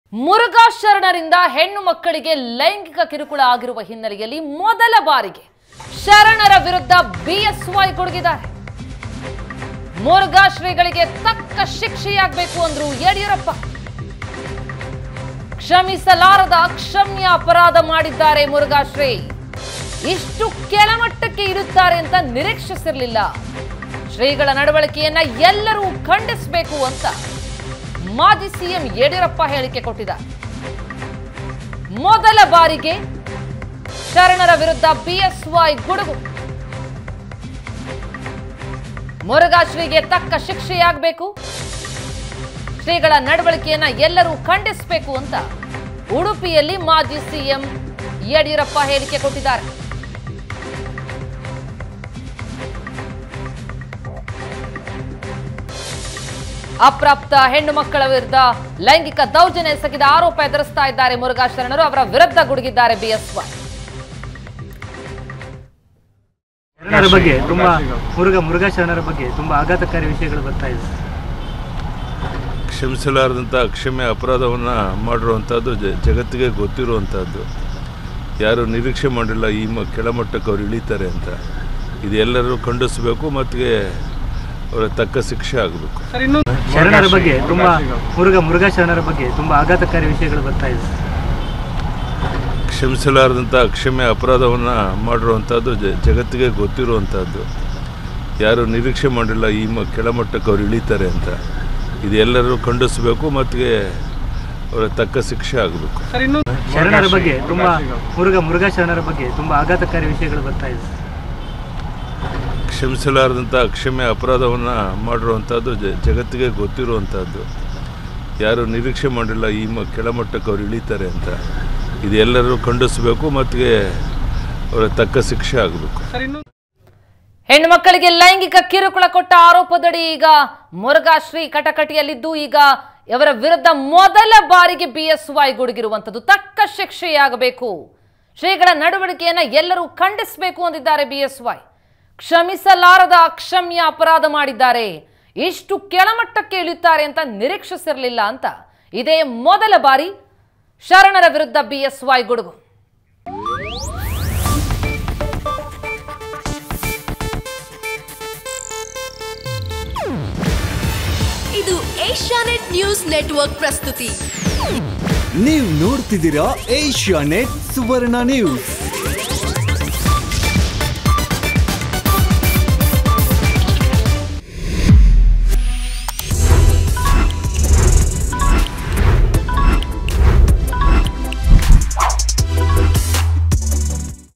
ಮುರುಘಾ ಶ್ರೀಗಳು ಇಷ್ಟು ಕೀಳು ಮಟ್ಟಕ್ಕೆ ಇಳಿಯುತ್ತಾರೆಂದು ನಿರೀಕ್ಷಿಸಿರಲಿಲ್ಲ | BS Yediyurappa |Murugha Mutt
ಮುರುಘಾ ಶ್ರೀಗಳು ಇಷ್ಟು ಕೀಳು ಮಟ್ಟಕ್ಕೆ ಇಳಿಯುತ್ತಾರೆಂದು ನಿರೀಕ್ಷಿರಲಿಲ್ಲ | BS Yediyurappa |Murugha Mutt Seer